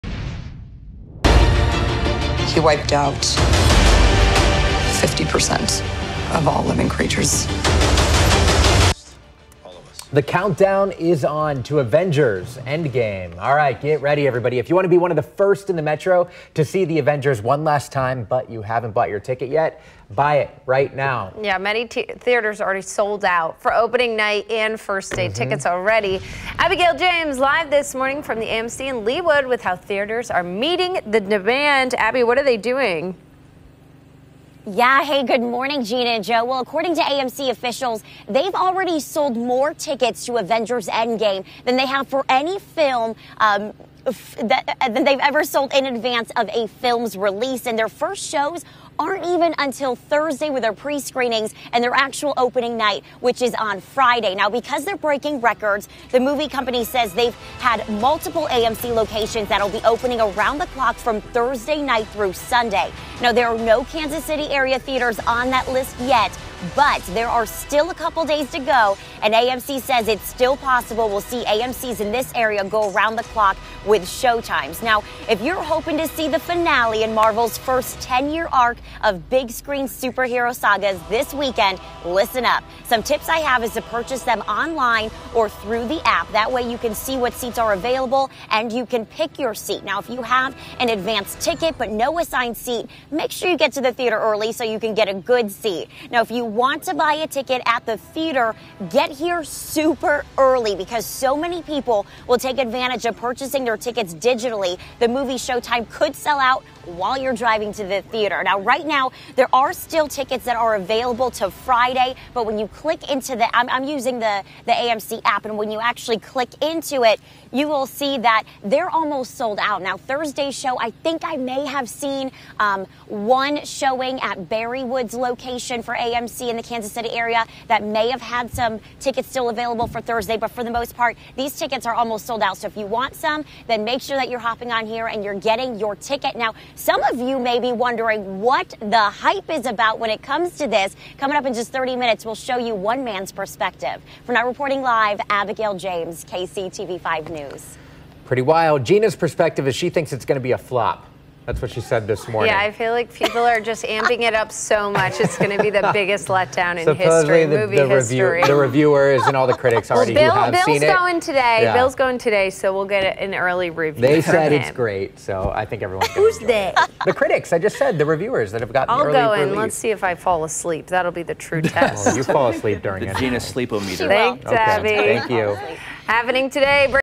Kansas City superhero fans prepare for "Avengers: Endgame"
If you’re hoping to be one of the first in the metro to see the Avengers assemble one last time but haven’t bought your ticket yet for Avengers: Endgame, you may want to do so quickly.